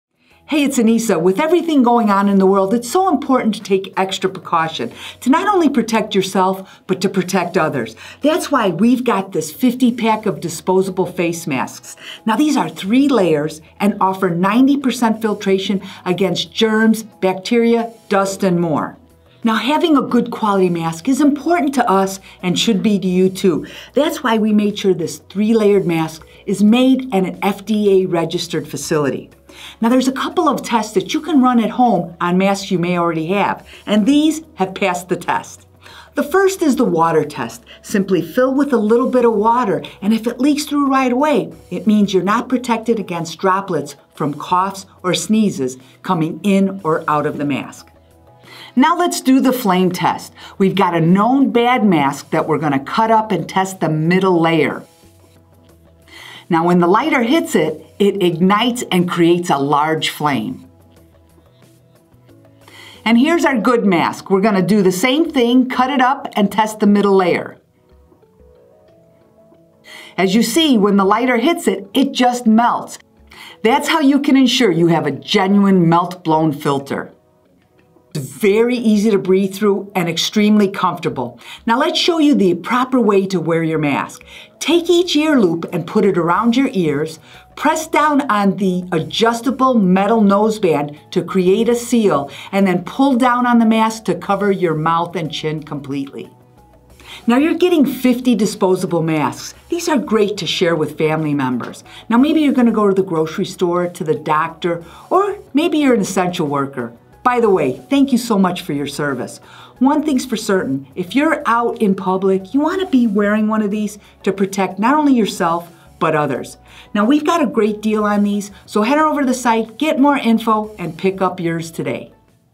Disposable Face Masks: 3 Layer Surgical-Style Non-Medical Masks with Water and UPDATED Flame Test
Stay safe with these 3-layer surgical face masks (non-medical) during flu and virus season. These are made in an FDA-Registered facility and are great general use masks to filter out over 90% of germs, smoke, droplets and more.

How can you tell if your masks are good? Make sure it will protect you from small droplets (from coughs and sneezes) by pouring water in the mask. If the water drips through right away - it's bad! Also carefully try the flame test: cut it open, and isolate the middle layer. Take a lighter and see if it creates a flame. Genuine melt-blown fabric simply melts instead!